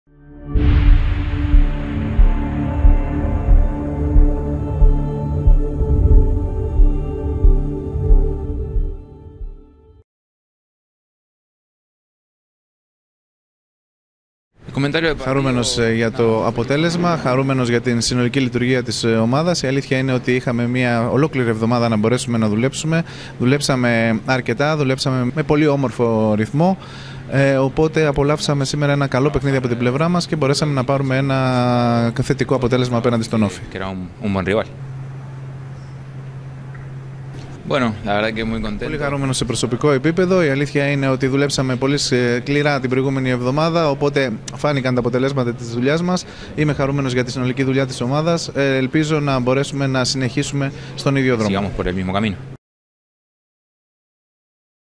Πόνσε: «Να συνεχίσουμε στον ίδιο δρόμο» | AEK F.C.
Ο Έκι Πόνσε αναδείχθηκε Cosmote MVP του αγώνα με τον ΟΦΗ, μέσα από την ψηφοφορία μεταξύ των μελών της επίσημης κοινότητας της ΠΑΕ ΑΕΚ στο Viber.

Ο Αργεντινός επιθετικός της ομάδας μας πέτυχε ακόμη δύο γκολ (έπειτα από το χατ τρικ απέναντι στον Ατρόμητο την προηγούμενη αγωνιστική) και ήταν καθοριστικός για την επικράτηση της ΑΕΚ.

Κατά την παραλαβή του βραβείου του, μίλησε στην κάμερα του AEK TV τόσο για την εξέλιξη του αγώνα, όσο και για την προσωπική του απόδοση…